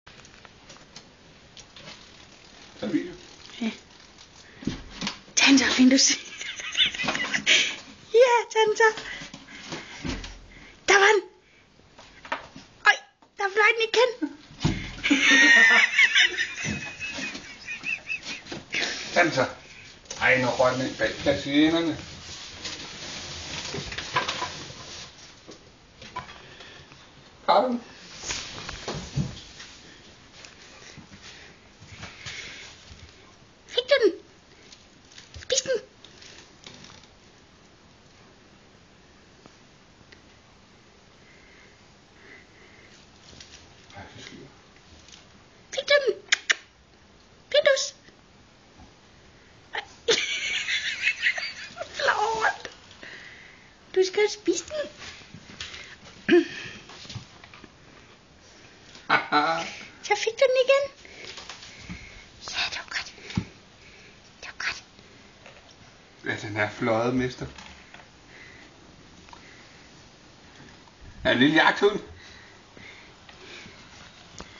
Findus fanger en natsværmer 11 august 09.MP4
Findus trying to catch a bug - 11 weeks old